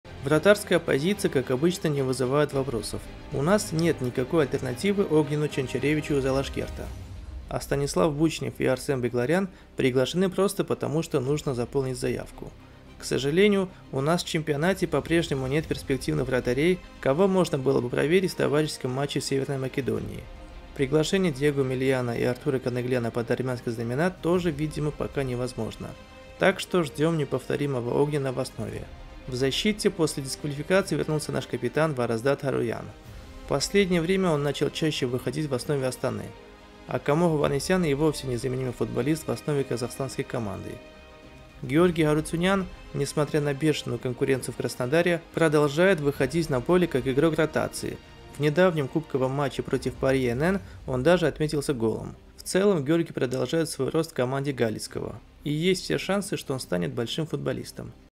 Заявка сборной Армении на матч против Латвии
Александр Петраков объявил список футболистов сборной Армении на матчи против Латвии и Северной Македонии